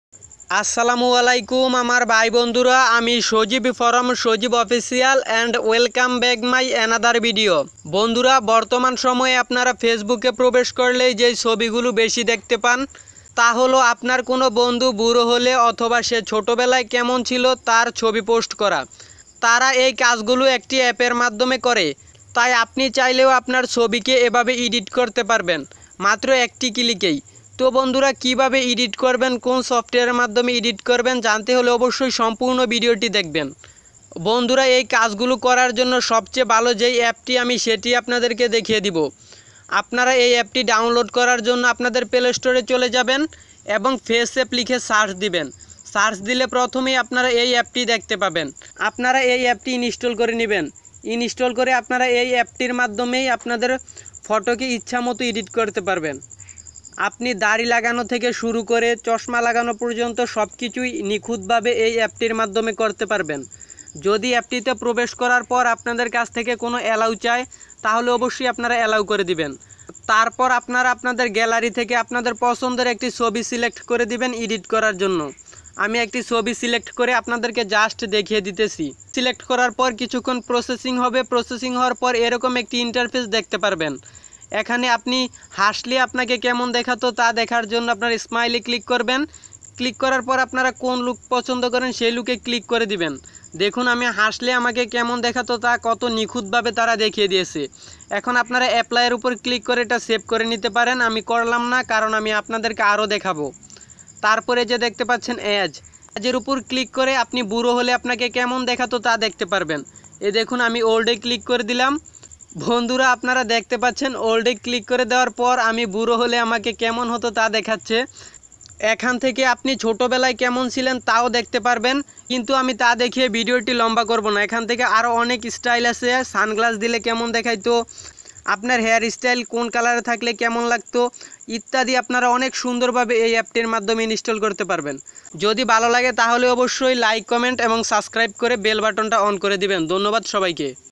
বুড়ো হলে কেমন কেমন দেখা যাবে | faceapp best age changeing app | Sajib Official
কের ভিডিওতে আমি আপনাদের সাথে একটি এডিটিং অ্যাপ শেয়ার করেছি। যেটার মাধ্যমে আপনারা দেখতে পারবেন যে, আপনারা বুড়ো হলে আপনাদের কেমন দেখাবে অথবা ছোটবেলায় আপনারা কেমন ছিলেন। এইসব ছবি এডিট করার জন্য বেস্ট সফটওয়্যার যার নাম হল ফেস অ্যাপ ( faceapp)
age changing app
About Us :Sajib Official is a Education YouTube Channel. You will find education tips videos in Bengali.